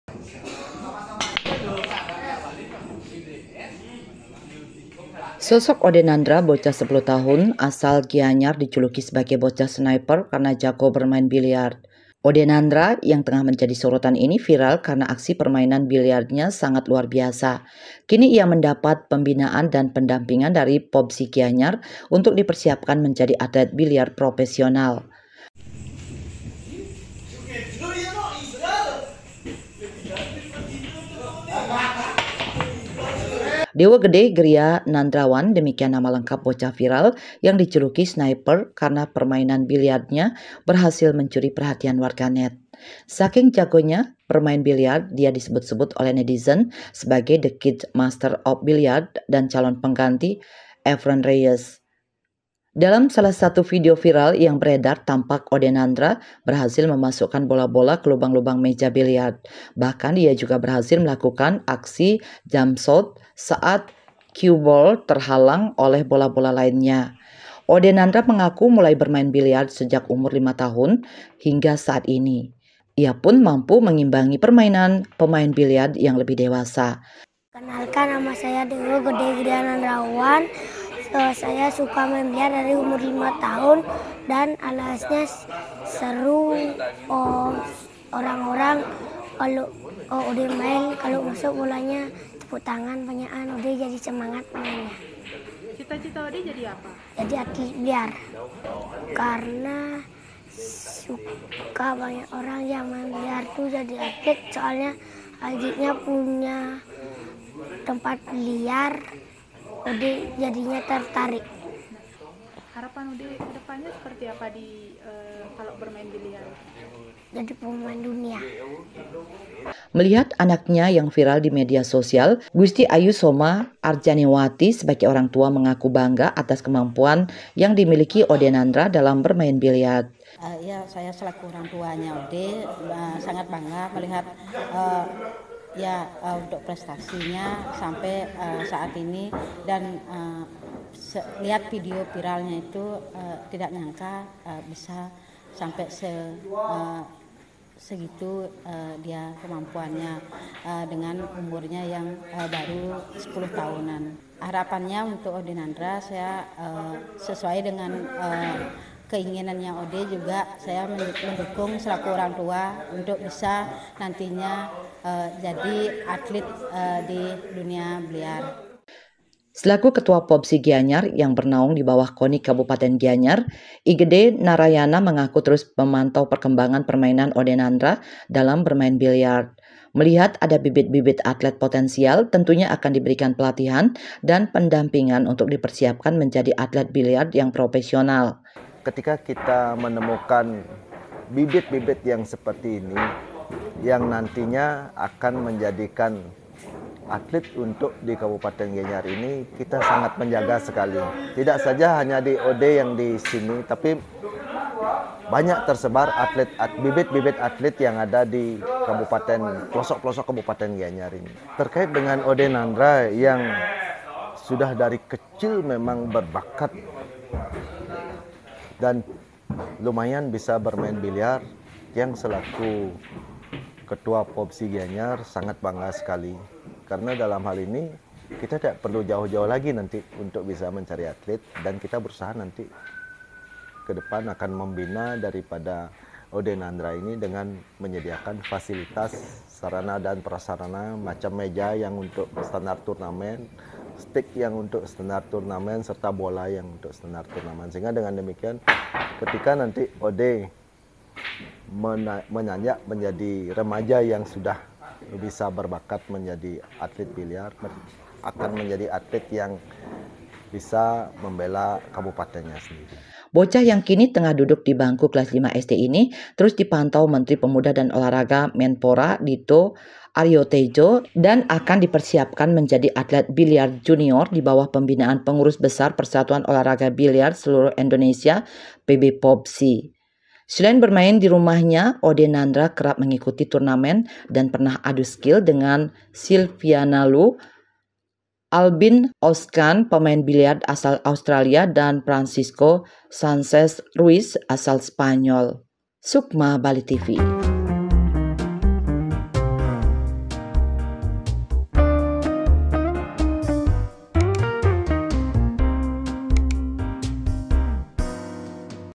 Wow! Bocah Sniper Asal Gianyar Ini, Pernah Adu skill Dengan Silviana Lu dan Albin Ouschan
Wow! Ode Nandra, bocah viral Sniper asal Gianyar. Ternyata sudah pernah mengikuti turnamen dan adu skill dengan Silviana Lu, Albin Ouschan pemain biliar asal Austria dan Francisco Sanchez Ruiz asal Spanyol